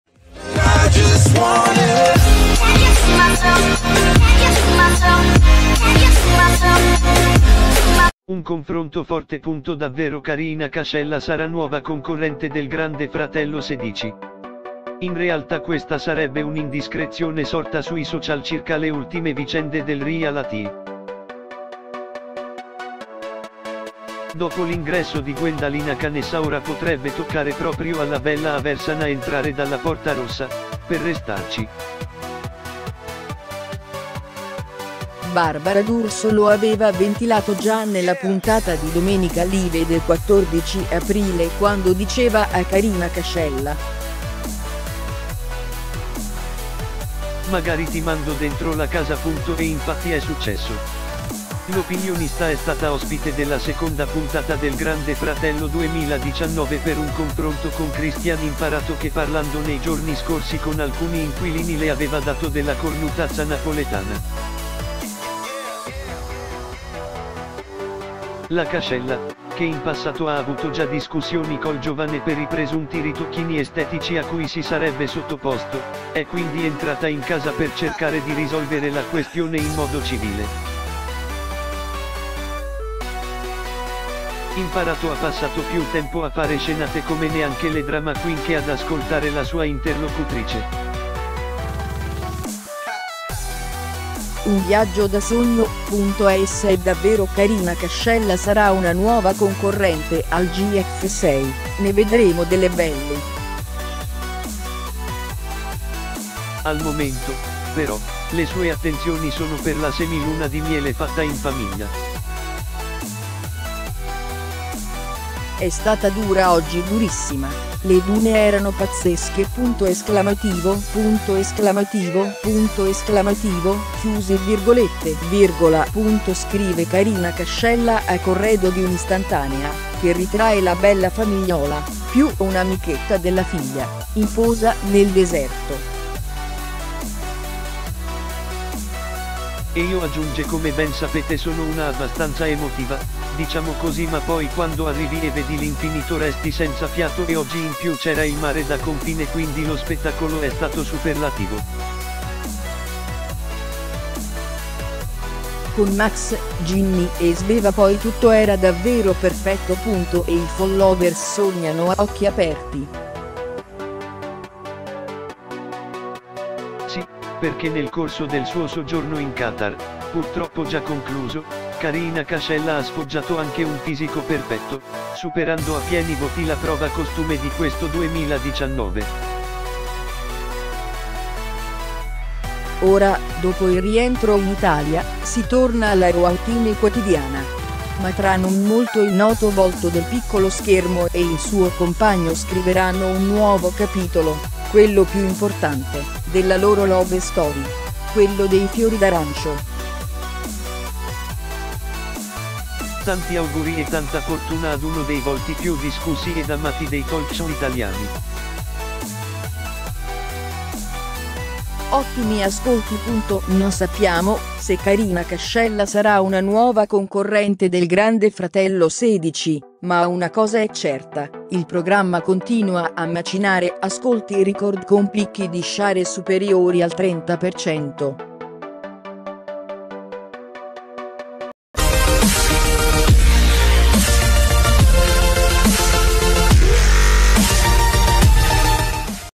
Grande Fratello 16: Karina Cascella nuova concorrente? Ecco la verità
Un confronto forte
Davvero Karina Cascella sarà nuova concorrente del Grande Fratello 16? In realtà questa sarebbe un’indiscrezione sorta sui social circa le ultime vicende del reality. Dopo l’ingresso di Guendalina Canessa ora potrebbe toccare proprio alla bella aversana entrare dalla porta rossa, per restarci. Barbara d’Urso lo aveva ventilato già nella puntata di Domenica Live del 14 aprile quando diceva a Karina Cascella:

“Magari ti mando dentro la casa”

e infatti è successo. L’opinionista è stata ospite della seconda puntata del Grande Fratello 2019 per un confronto con Cristian Imparato che parlando nei giorni scorsi con alcuni inquilini le aveva dato della “cornutazza napoletana”.

La Cascella, che in passato ha avuto già discussioni col giovane per i presunti ritocchini estetici a cui si sarebbe sottoposto, è quindi entrata in casa per cercare di risolvere la questione in modo civile. Imparato ha passato più tempo a fare scenate come neanche le drama queen che ad ascoltare la sua interlocutrice.

Un viaggio da sogno
Se davvero Karina Cascella sarà una nuova concorrente al GF 6, ne vedremo delle belle. Al momento, però, le sue attenzioni sono per la semi luna di miele fatta in famiglia.

“È stata dura oggi durissima, le dune erano pazzesche!!!”,

scrive Karina Cascella a corredo di un’istantanea che ritrae la bella famigliola (più un’amichetta della figlia) in posa nel deserto.

“E io – aggiunge – come ben sapete sono una abbastanza emotiva… diciamo così ma poi quando arrivi e vedi l’infinito resti senza fiato e oggi in più c’era il mare da confine quindi lo spettacolo è stato superlativo. Con Max, Ginni e Sveva poi tutto era davvero perfetto”.

E i followers sognano a occhi aperti.

Sì, perché nel corso del suo soggiorno in Qatar (purtroppo già concluso) Karina Cascella ha sfoggiato anche un fisico perfetto, superando a pieni voti la prova costume di questo 2019. Ora, dopo il rientro in Italia, si torna alla routine quotidiana: ma tra non molto il noto volto del piccolo schermo e il suo compagno scriveranno un nuovo capitolo – quello più importante – della loro love story: quello dei fiori d’arancio. Tanti auguri e tanta fortuna ad uno dei volti più discussi ed amati dei talk show italiani.

Ottimi ascolti
Non sappiamo se Karina Cascella sarà una nuova concorrente del Grande Fratello 16, ma una cosa è certa, il programma continua a macinare ascolti record con picchi di share superiori al 30%.